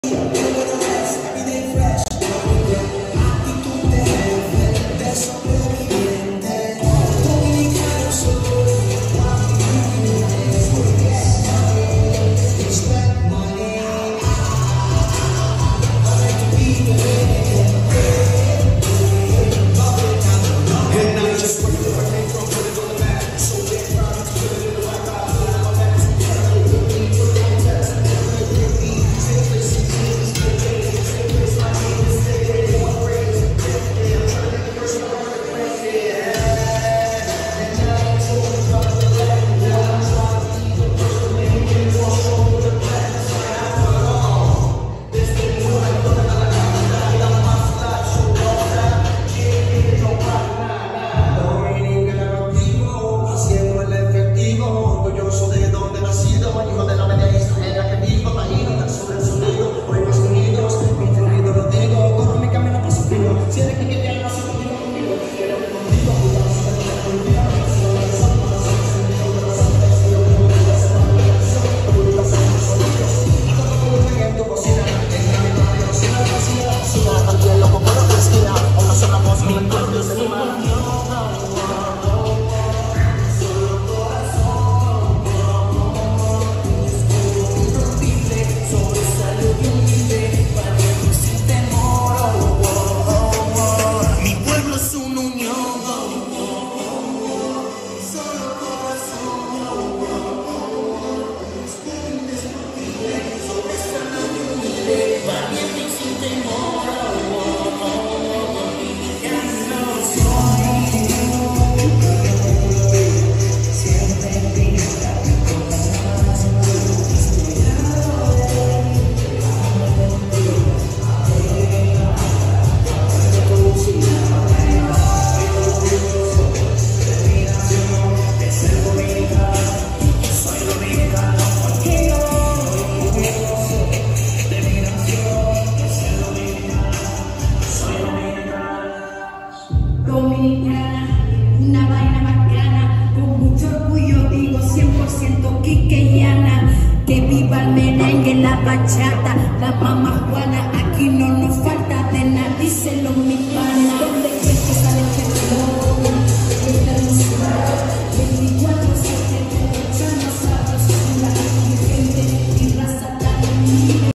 La Casa Dominicana celebra Independencia Dominicana 2023 en Liga de Arte
La Casa Dominicana de Hazleton Pennsylvania celebra la Independencia Dominicana en